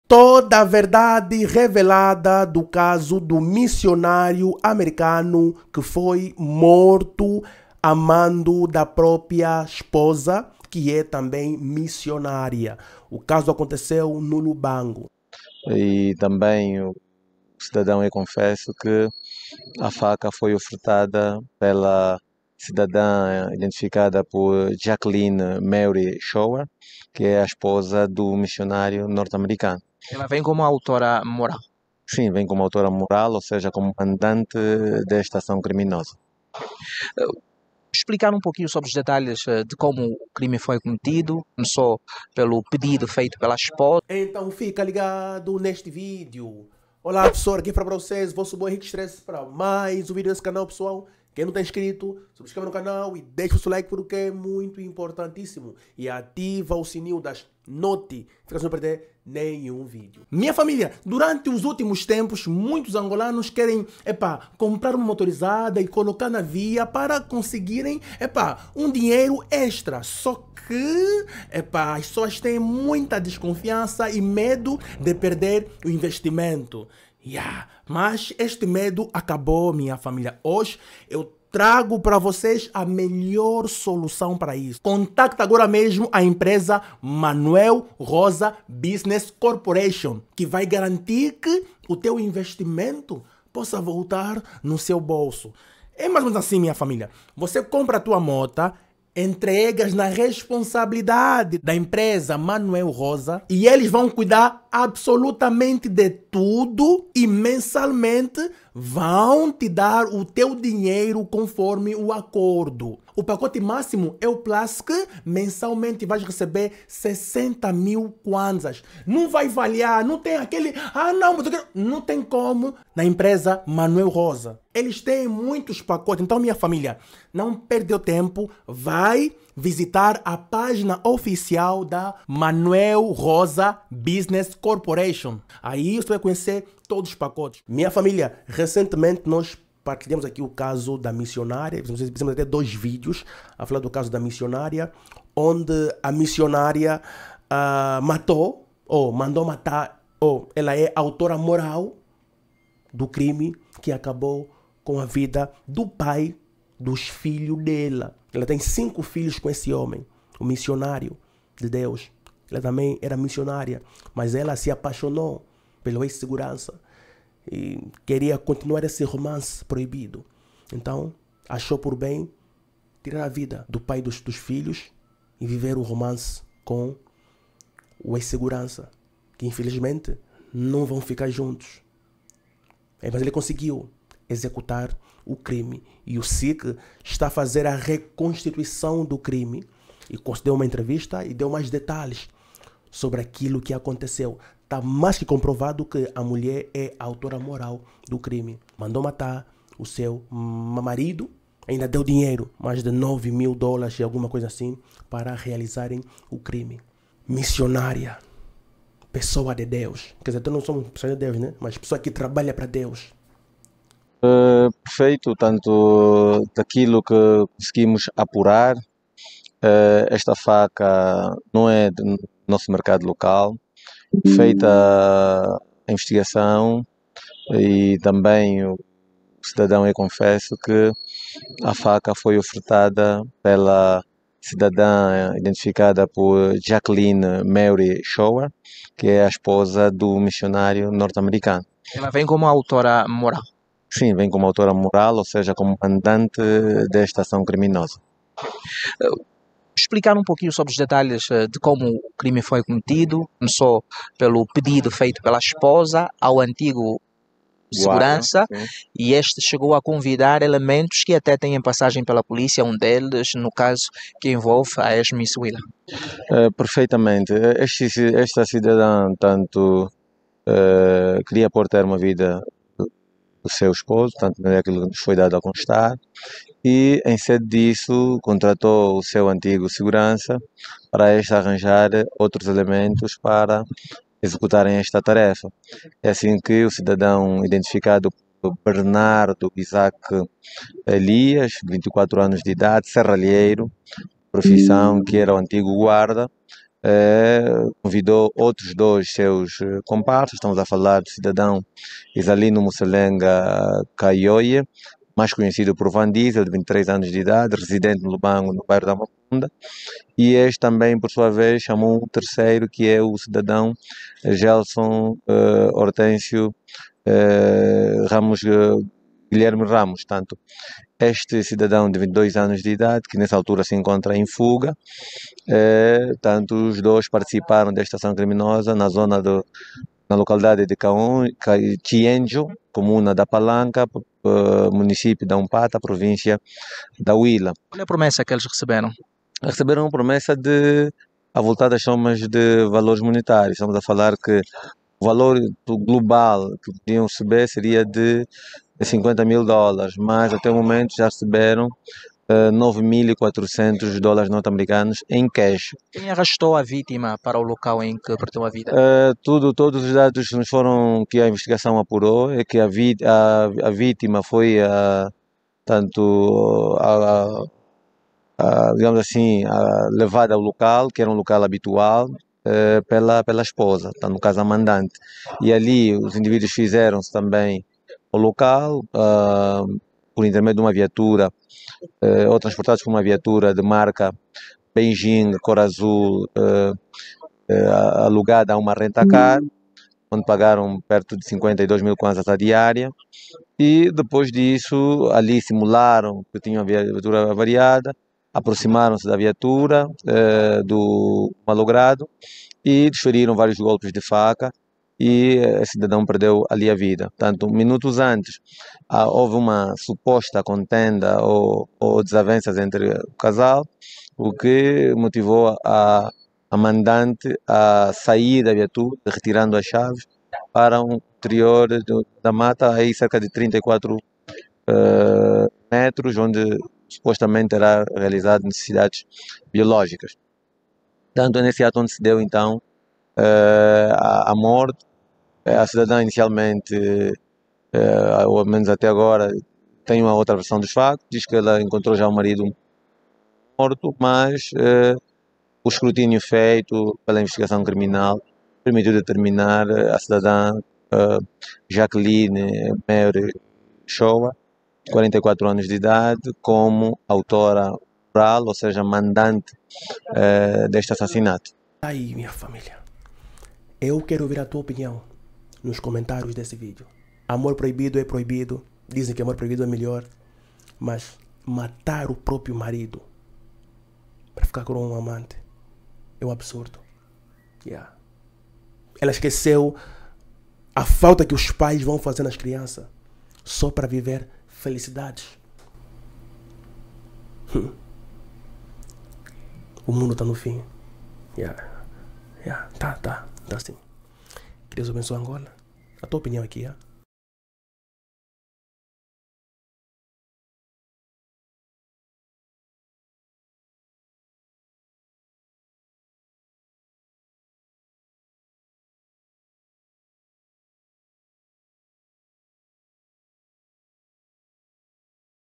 Toda a verdade revelada sobre a missionária que matou o marido
Contacto Profissional
Banco Sol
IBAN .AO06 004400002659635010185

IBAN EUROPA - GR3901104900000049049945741
faca a sua (doação) para ajudar o canal